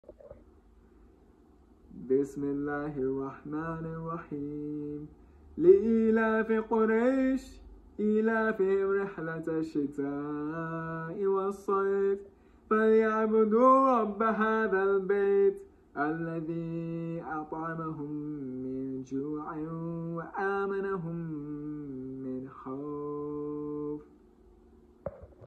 Quraysh (106)
I try my best reciting, but let me know if I've made any mistakes I've missed :)